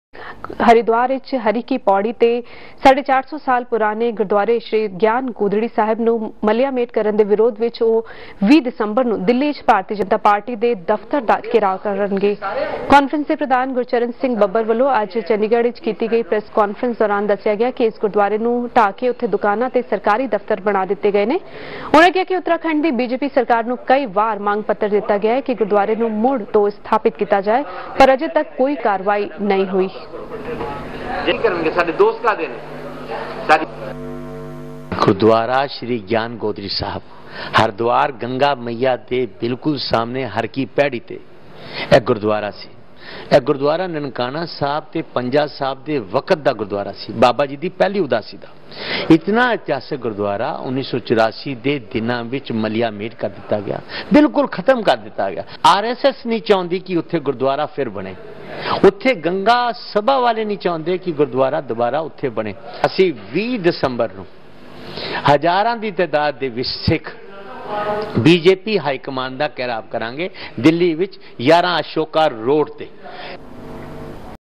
Sikhs to protest at BJP HQ on 20 December,2011
Sikhs will protest at the BJP Headquarters in New Delhi on 20 December,2011 in protest against demolition of Gurdwara Gyan Godhrri Sahib,Haridwar by RSS in 1984.

This protest which is to be held under the leadership of Baba Baljit Singh Daduwaal and All India Sikh Conference head Gurcharan Singh Babbar is also to free the present Gurdwara land from under RSS control.